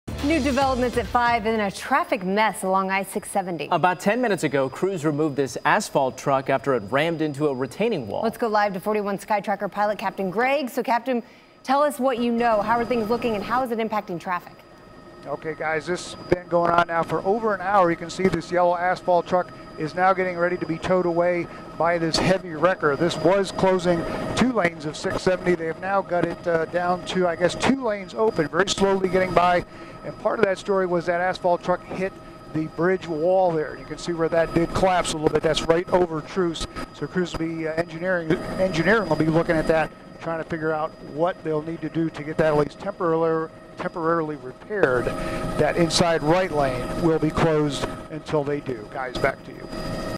Asphalt truck hits barrier on WB I-70
An asphalt truck hit a jersey barrier on westbound I-70 over Troost Ave. Tuesday afternoon.